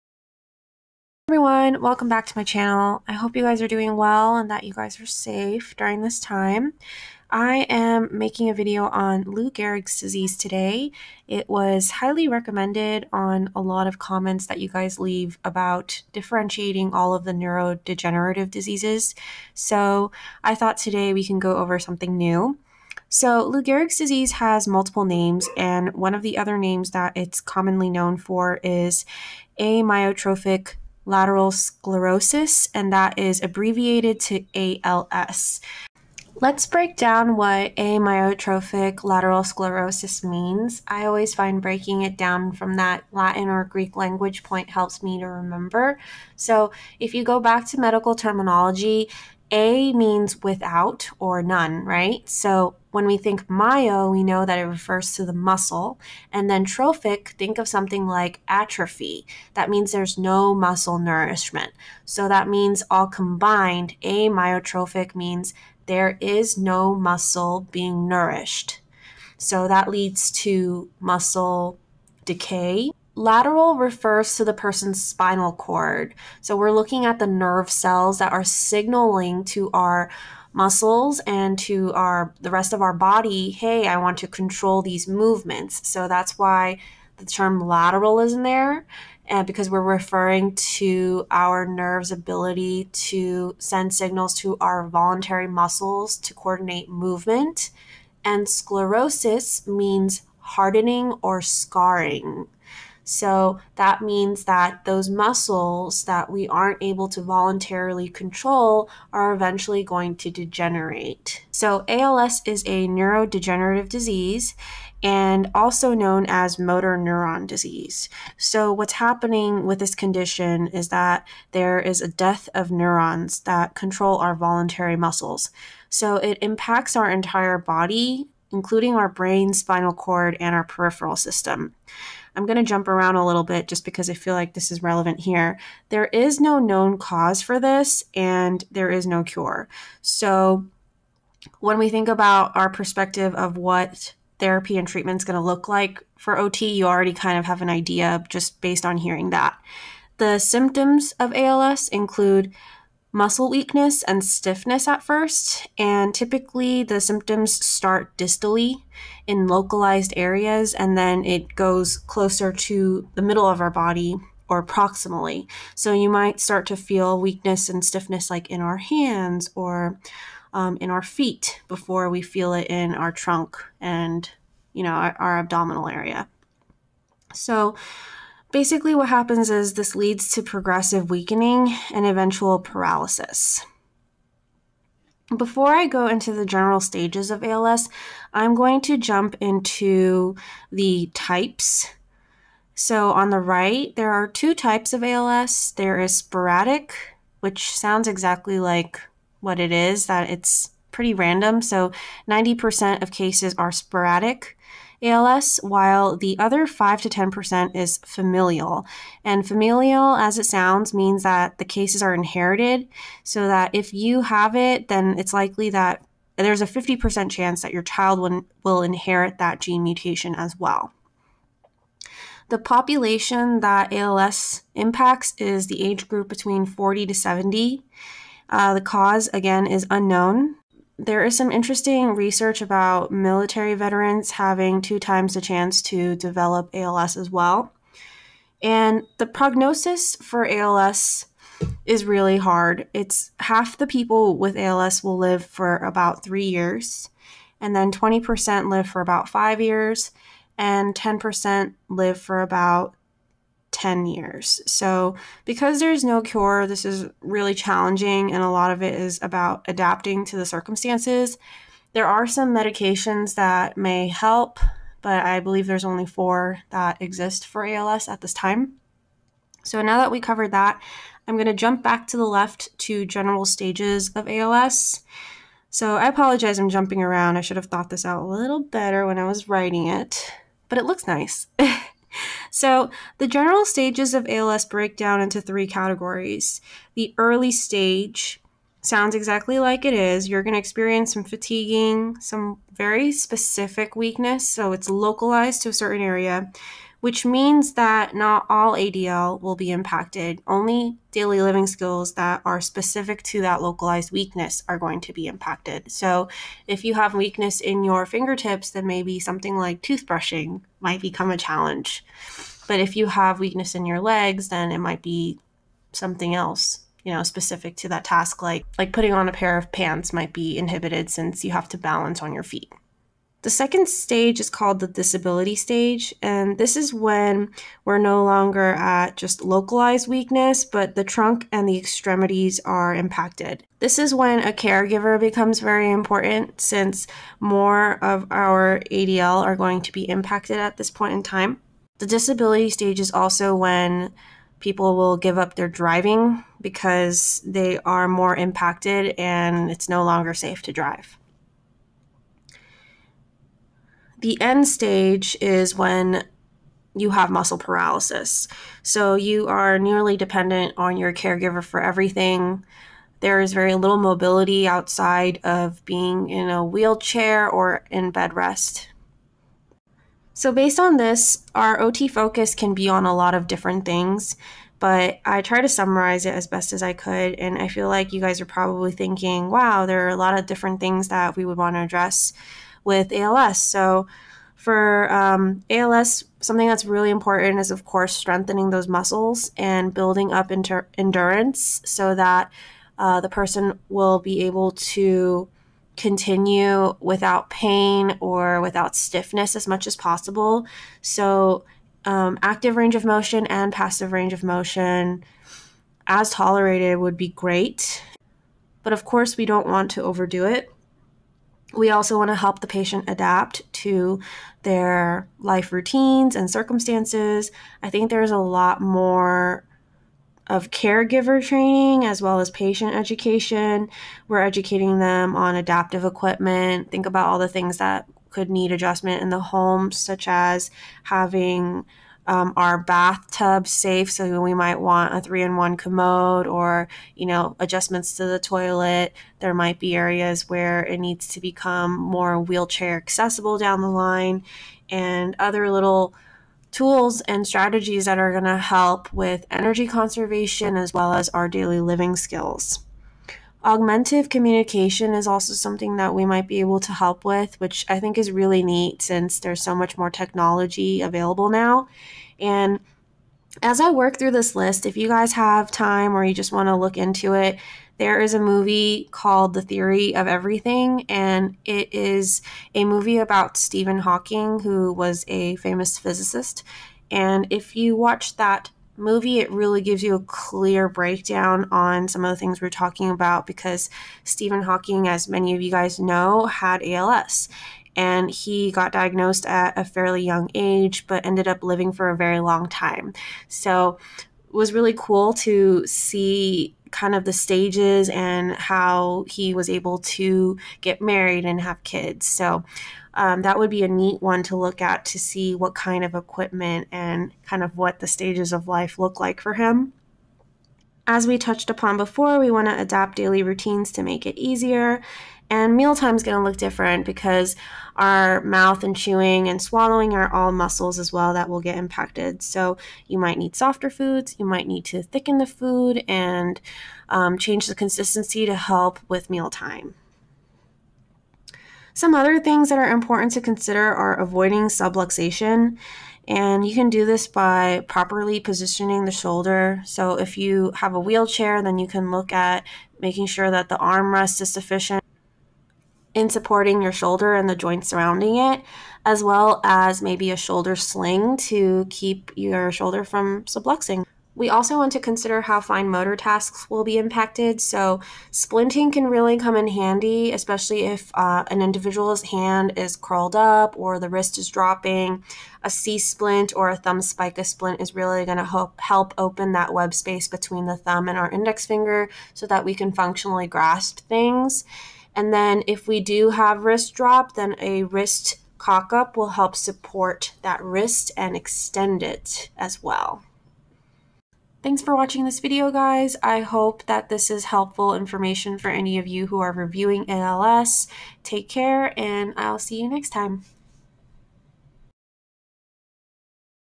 OT Rex - Amyotrophic Lateral Sclerosis (ALS) Review
Hey everyone!

I'm getting some more neurodegenerative disorder reviews out this week. This video is pretty straightforward. All pictures are from google and do not belong to me!